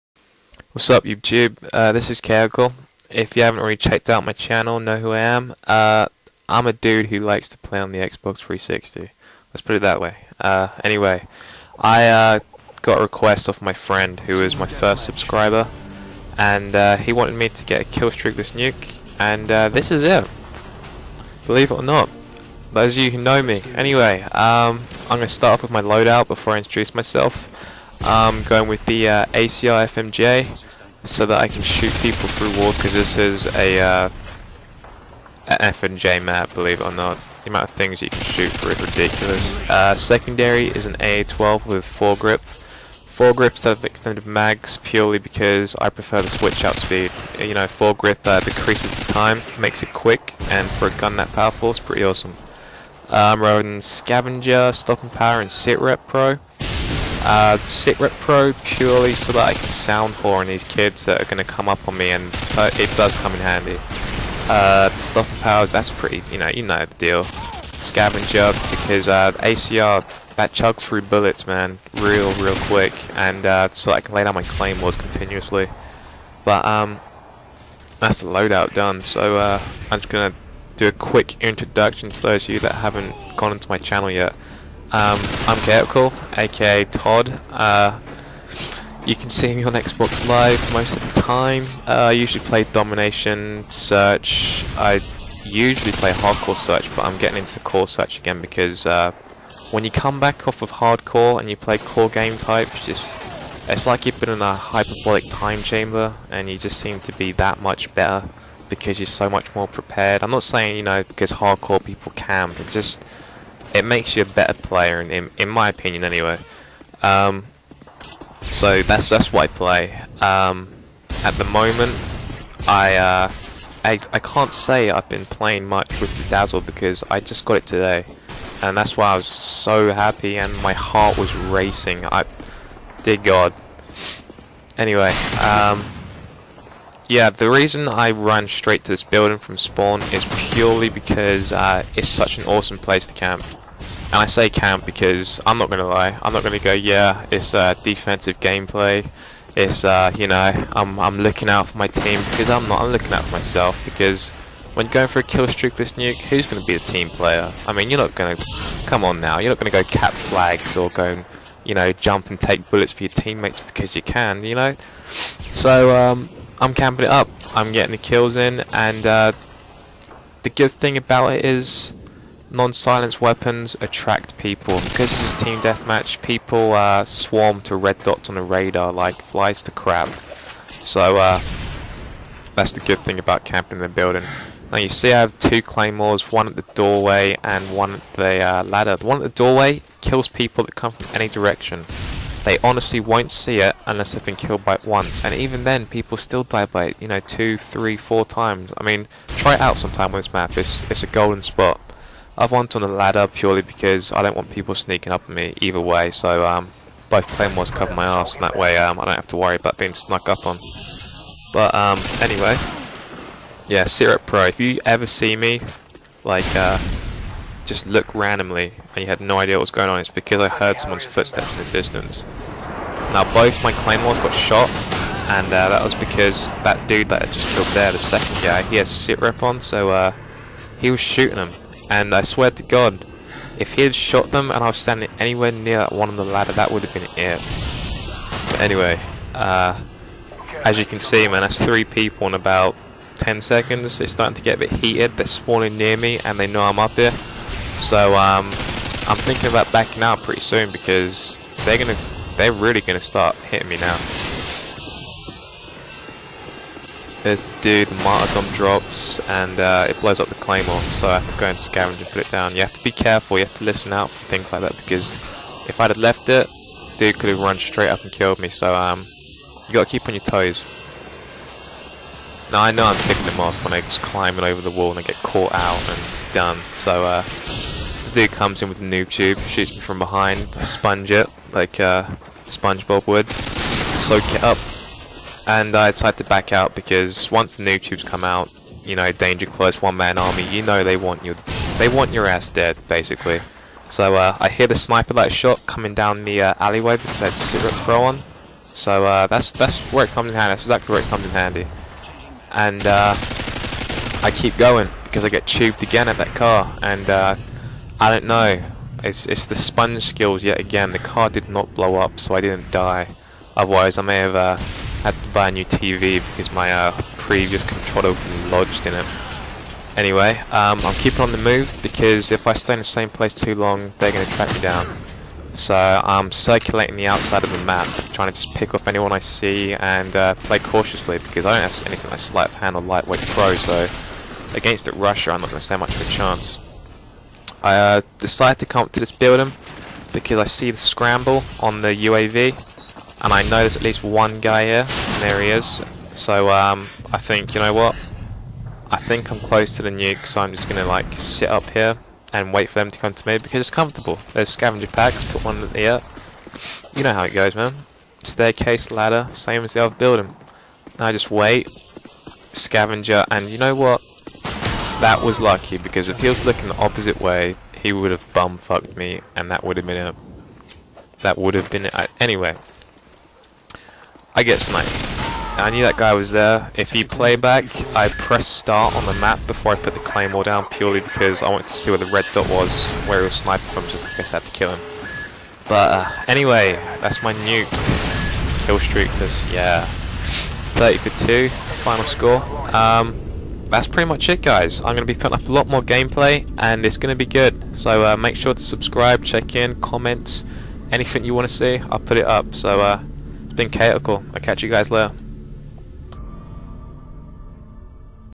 Khaoticall's Killstreakless Nuke
In advance i'm sorry about the video quality, this is my first actual gameplay video and it was captured within 1 hour or me putting my dazzle all together.